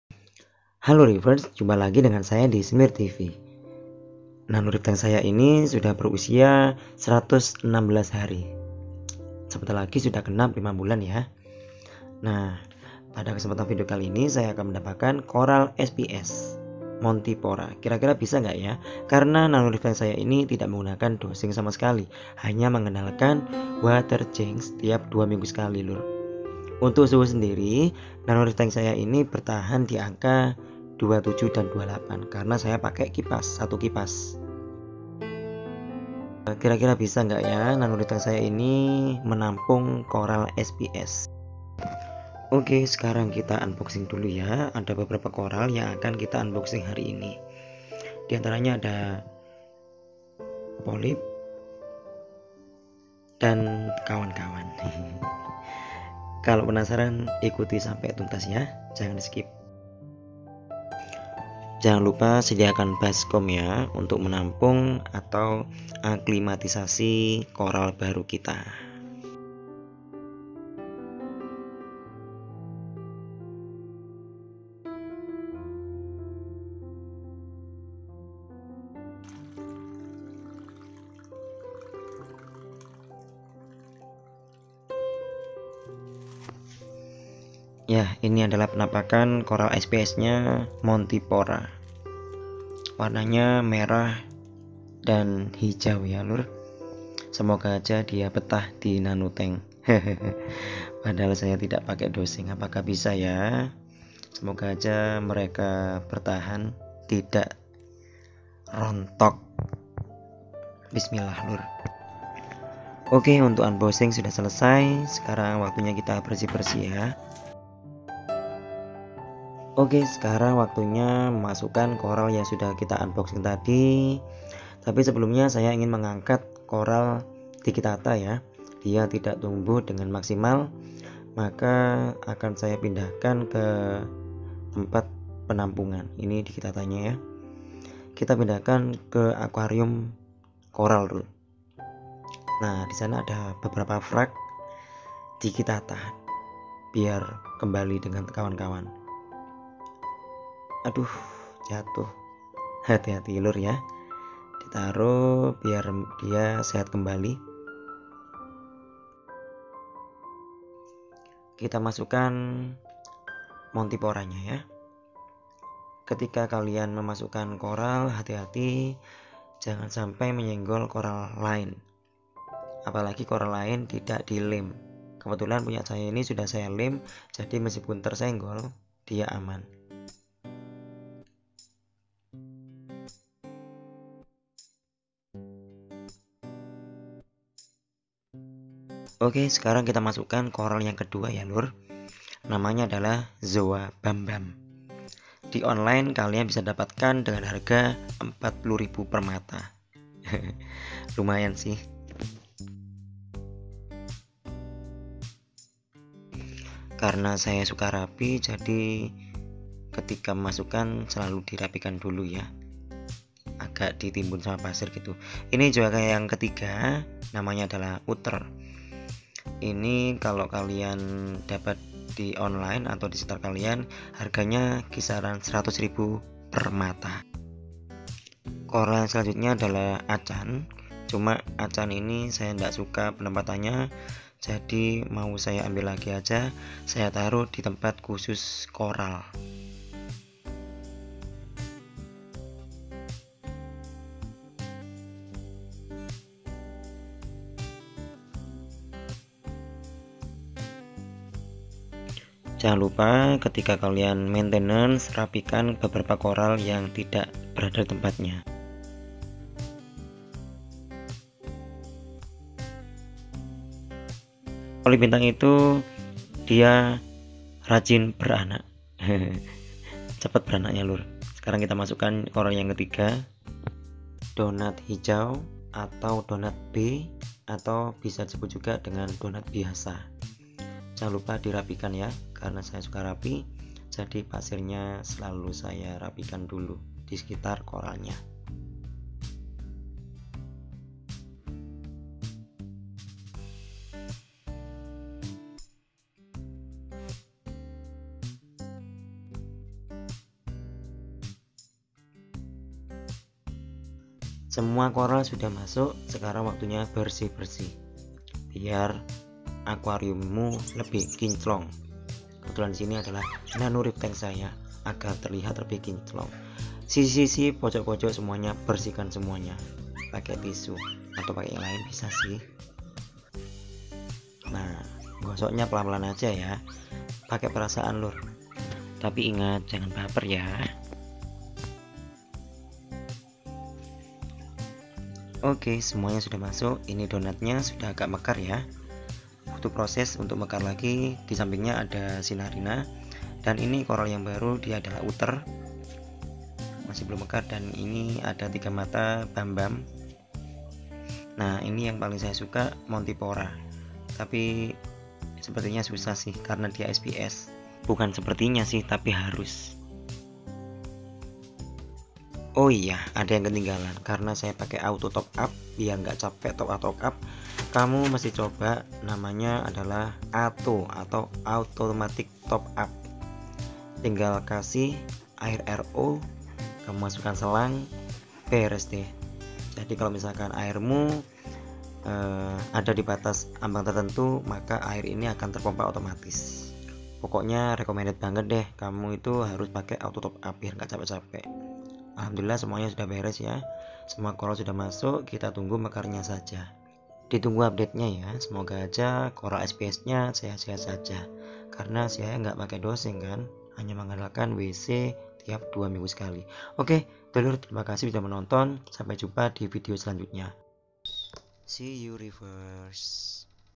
Nano Reef Tank Without Dose Add SPS Coral, Will it Last?
Nano Reef Tank 10 Gallon: Add SPS Coral, will it last?

hello reefer

The nano tank is 116 days old. let's try to add sps coral (montipora), can we? because my nano tank does not use a dose. only rely on Water Changes routine every 2 weeks. aquarium temperature 27-28°C. I am not use a skimmer (no skimmer)/ skimmerless

see also:
DIY Lamp Review, good but.